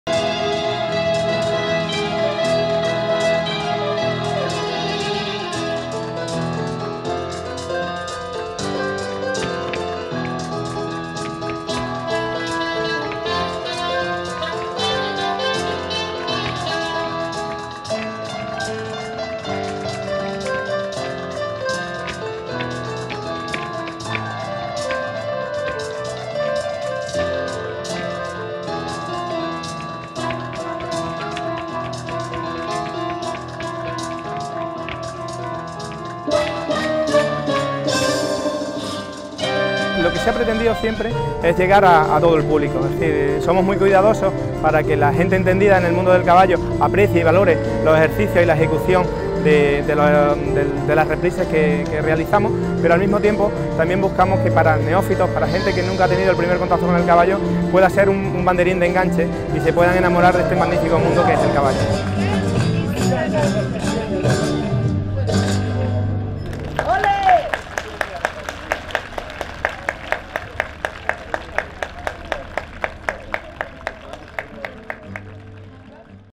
Exhibición ecuestre en la Plaza Roja de Moscú
La Asociación Ecuestre de Córdoba ofreció una extraordinaria exhibición con caballos y danza en la Plaza Roja de Moscú, en el marco del Festival Spasskaya Vashna 2018.